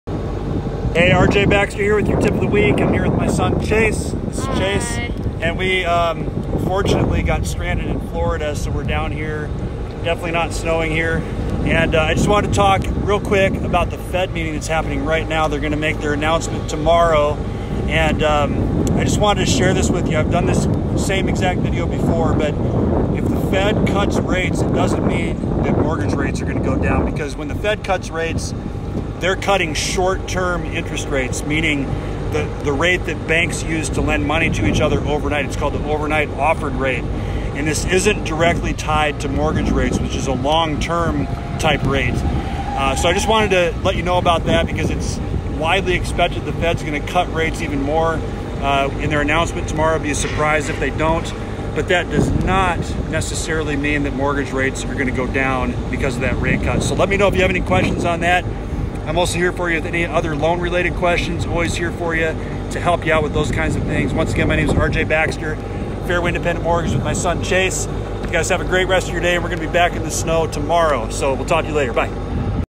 How are mortgages rate affected by rate cuts? | Tip of the week
The next Federal Reserve meeting is happening today and tomorrow.  If they cut rates what does it mean for mortgage rates?